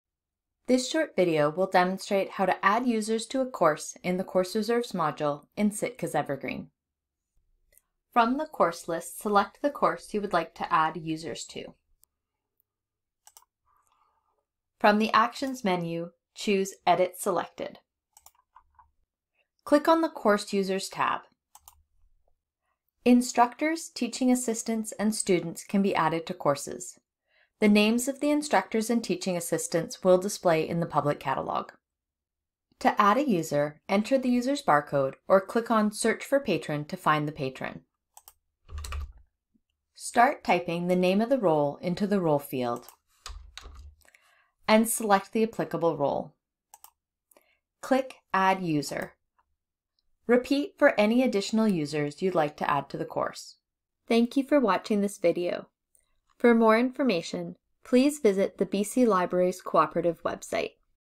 Sitka Snippet Video - Adding Users to a Course
This short video will demonstrate how to add users to a course in the course reserves module in Sitka’s Evergreen.